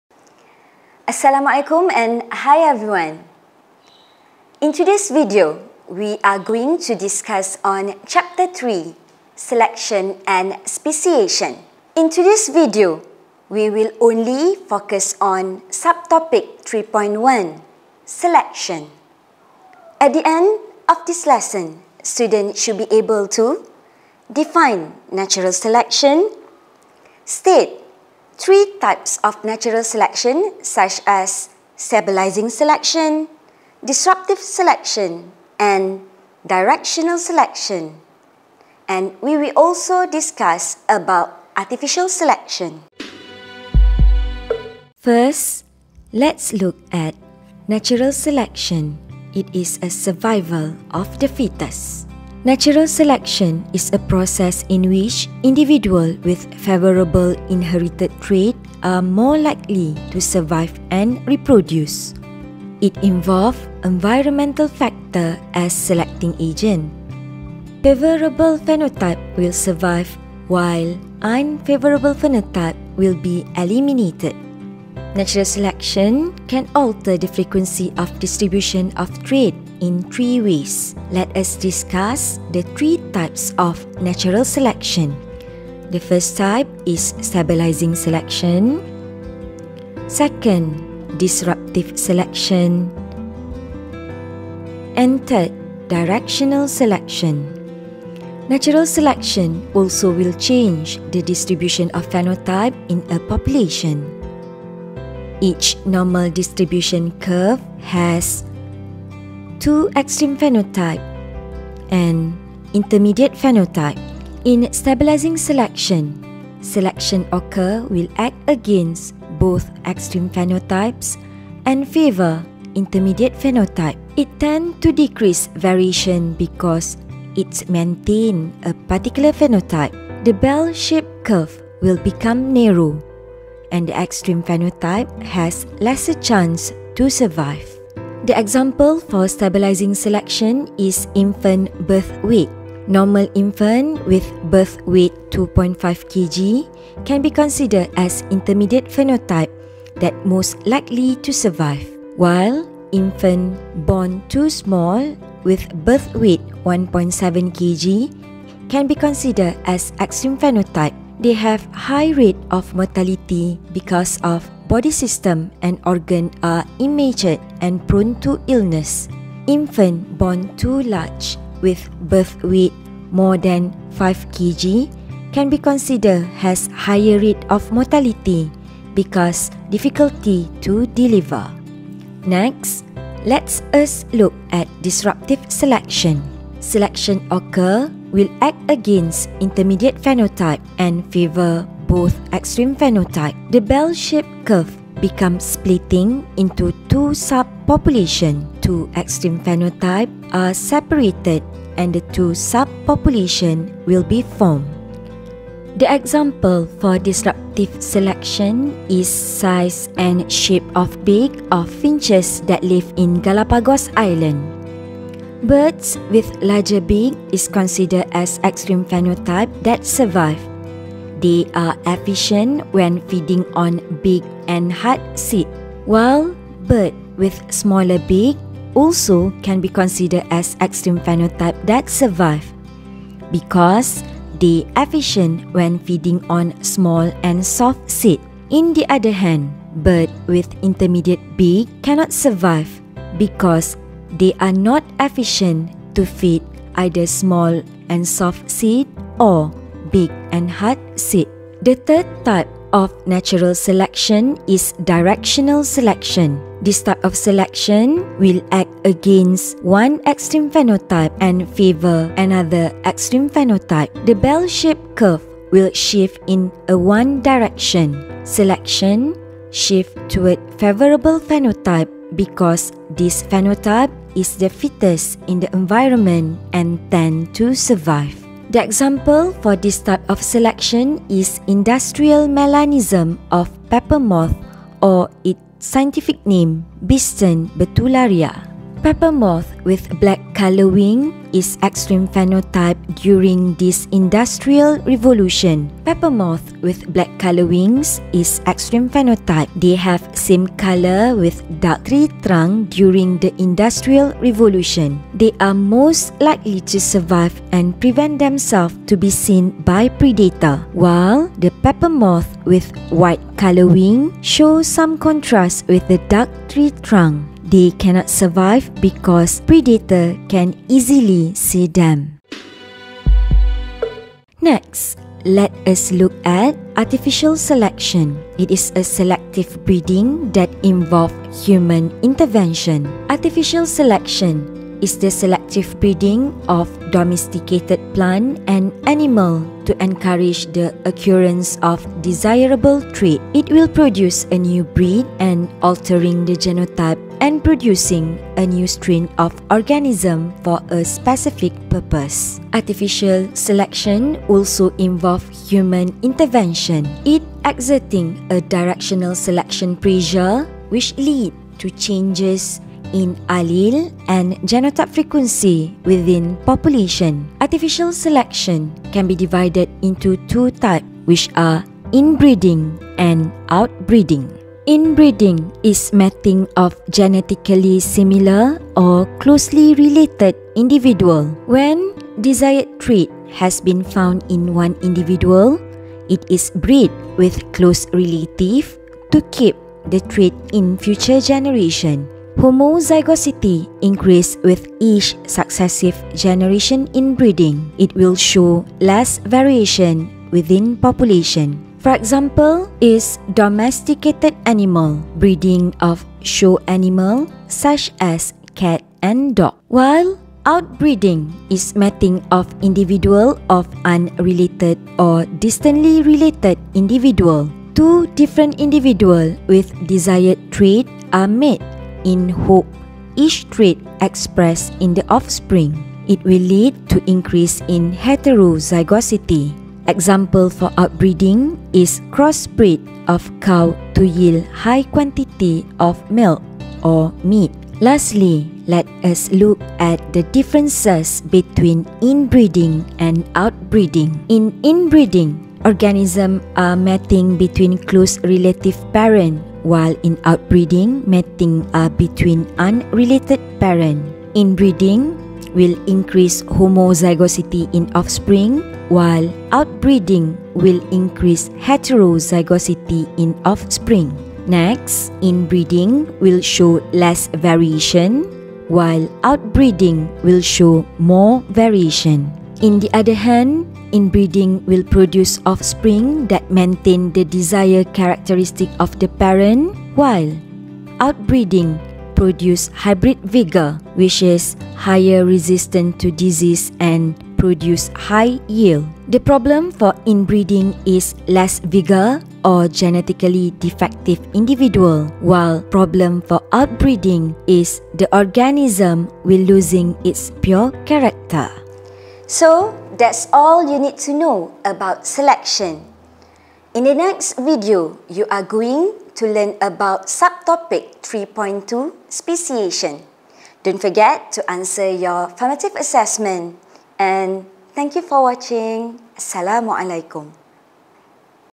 [LECT C3 : SELECTION & SPECIATION] 3.1 Selection
In this video, students will learn about two type of selection which are natural selection and artificial selection.
Detail explanation on three type of natural selection, two type of artificial selection and related example also included.